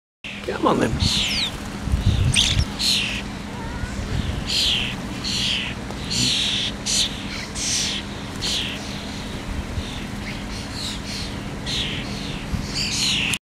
Goodness me, the starlings show a lot of enthusiasm at feeding time.. ;) :) :)